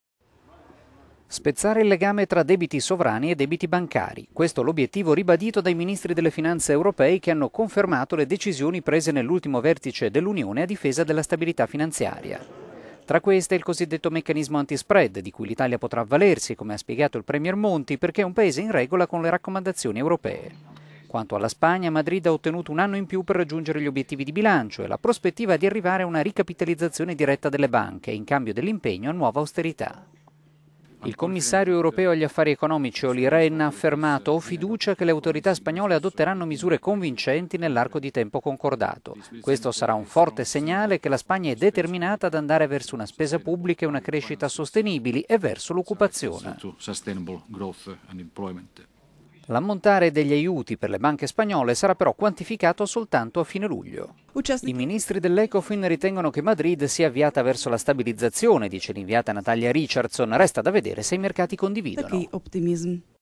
Ecofin e Eurogruppo muovono passi verso stabilità finanziaria
Spezzare il legame tra debiti sovrani e debiti bancari: questo l'obiettivo ribadito dai ministri delle finanze europei, che hanno confermato le decisioni prese nell'ultimo vertice dell'Unione a difesa della stabilità finanziaria.

Tra queste, il cosidetto meccanismo anti-spread, di cui l'Italia potrà avvalersi, come ha spiegato il premier Monti, perché è un paese in regola con le raccomandazioni europee. Quanto alla Spagna, Madrid ha ottenuto un anno in più per raggiungere gli obiettivi di bilancio e anche la prospettiva di arrivare a una ricapitalizzazione diretta delle banche, in cambio dell'impegno a nuova austerità.

Il commissario europeo agli affari Economici Olli Rehn: "Ho fiducia che le autorità spagnole adotteranno misure convincenti, nell'arco di tempo concordato. Questo sarà un forte segnale che la Spagna è determinata ad andare verso una spesa pubblica e una crescita sostenibili e verso l'occupazione".

L'ammontare degli aiuti per le banche spagnole sarà però quantificato soltanto a fine luglio. La corrispondente a Bruxelles, Natalia Richardson: "I ministri dell'Ecofin ritengono che Madrid sia avviata verso la stabilizzazione. Resta da vedere se i mercati condividono".